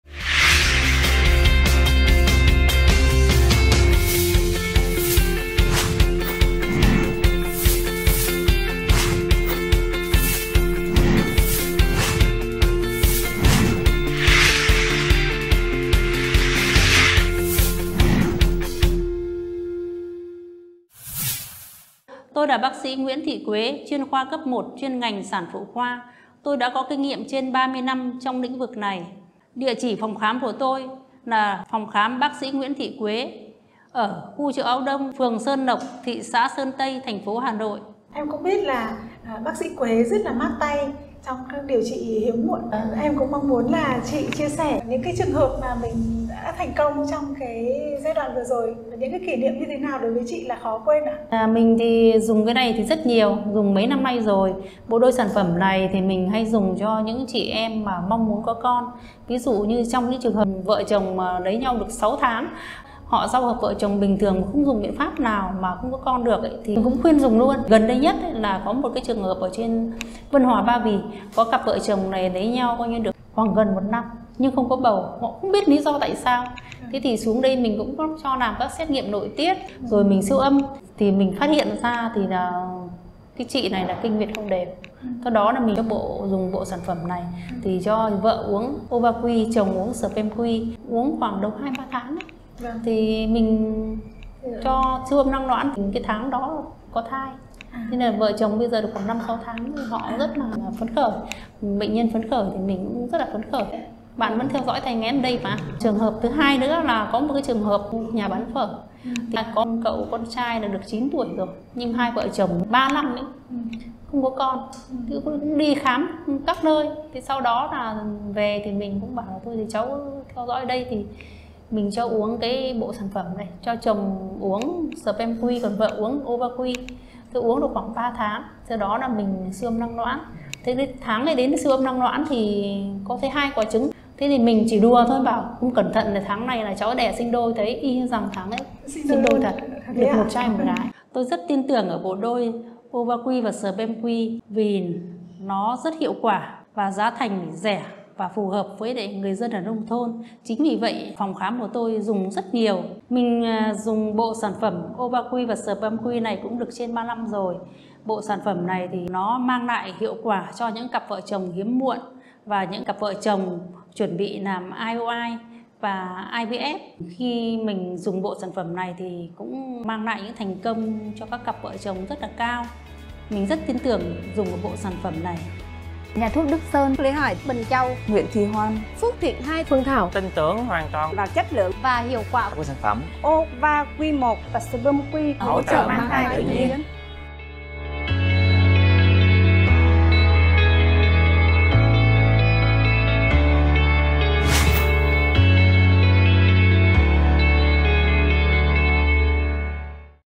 [Bs.CK Cấp 1] Bs. Nguyễn Thị Quế, Chuyên Ngành Sản Phụ Khoa, Có 30 Năm Kinh Nghiệm Chữa Vô Sinh
Vợ chồng THẢ 3 năm,  BỖNG MANG THAI ĐÔI nhờ OvaQ1, SpermQ,  Bs. Nguyễn Thị Quế tin tưởng bộ đôi này.
Cùng lắng nghe những chia sẻ về bộ đôi OvaQ1, SpermQ và câu chuyện của cặp vợ chồng mang thai đôi thành công.
OvaQ1, SpermQ dùng cho các cặp vợ chồng bị:
- KInh nguyệt không đều
- Đa nang, trứng kém
- Tinh trùng yếu, dị dạng

👉👉Comment ngay tại đây để được chuyên gia tư vấn trực tiếp
Xem thêm các thông tin khác về OvaQ1, SpermQ